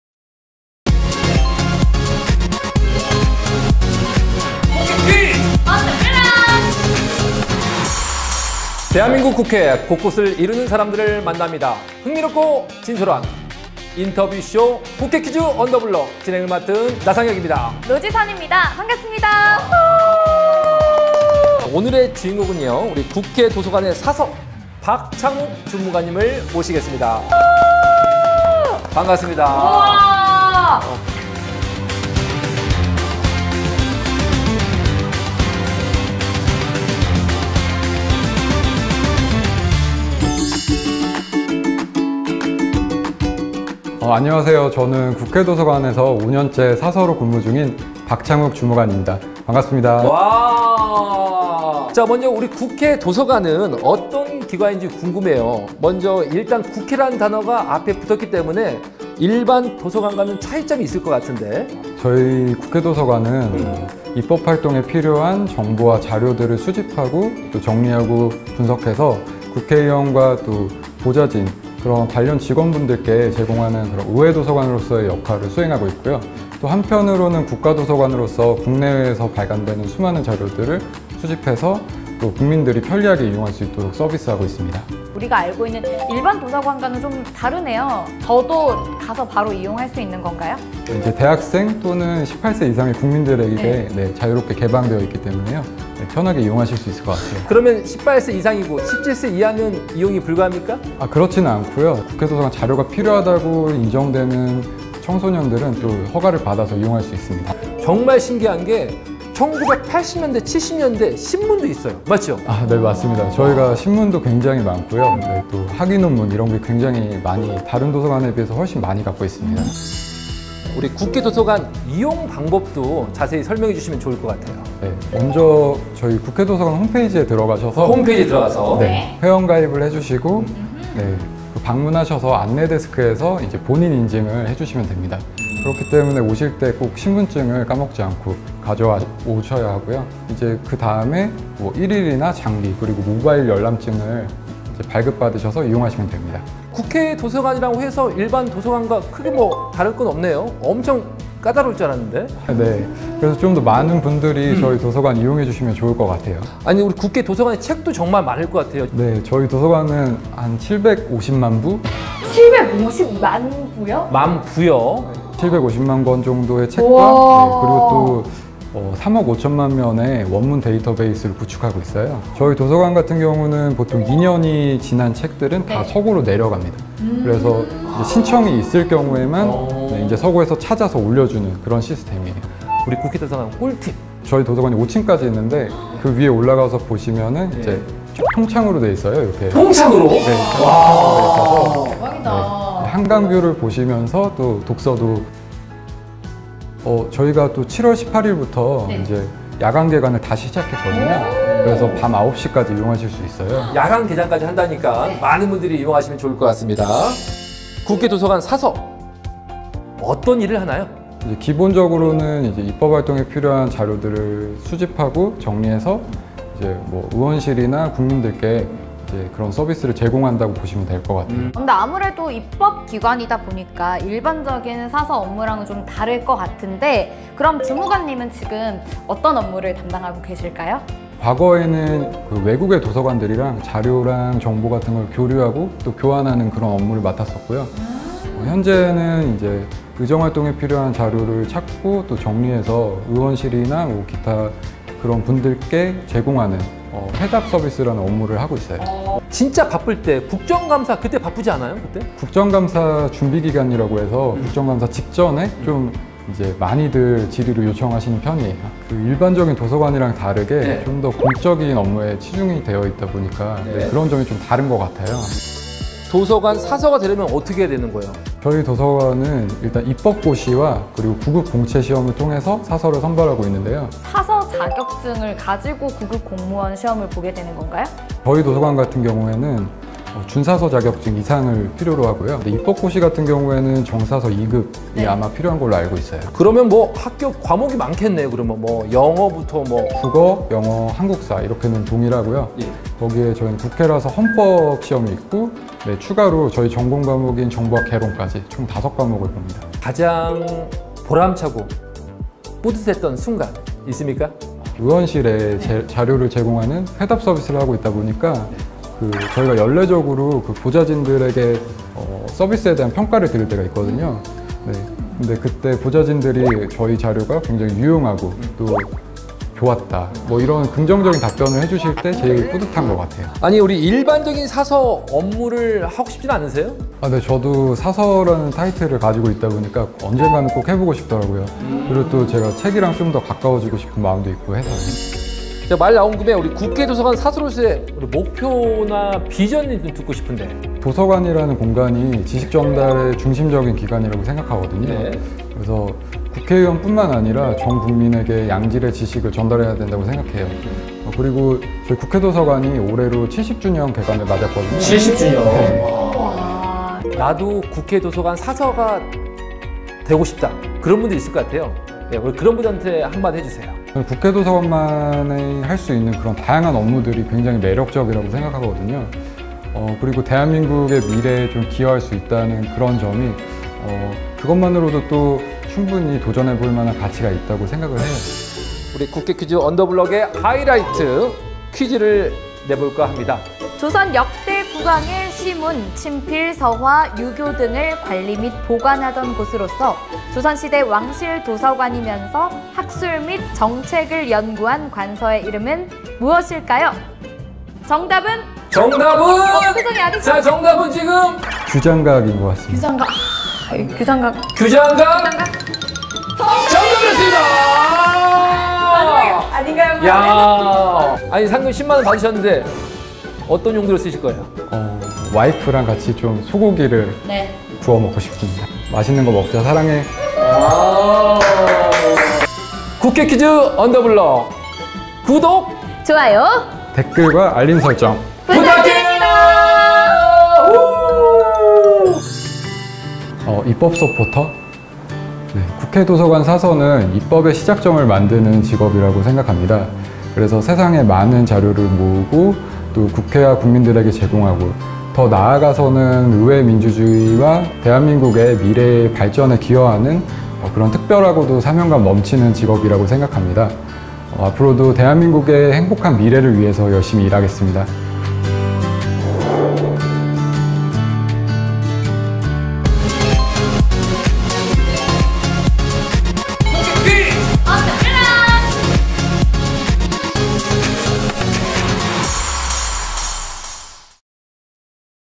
국회도서관에 가면 한강뷰를 즐길 수 있다?
당신이 지금까지 알지 못했던 국회 곳곳의 사람들의 이야기!
'국회퀴즈 온 더 블럭' 제 2편!

그 두 번째 주인공!
국회도서관 사서 박창욱 주무관을 만나봤는데요.

한강뷰를 보며 독서를 즐기고~
숨겨진 맛집 탐방까지 하고 싶다면?!
오직 [국회 퀴즈 온 더 블럭]에서만 만나볼 수 있는 국회도서관 이용 꿀팁!
지금 바로 공개합니다!

출처 : 대한민국 국회 공식채널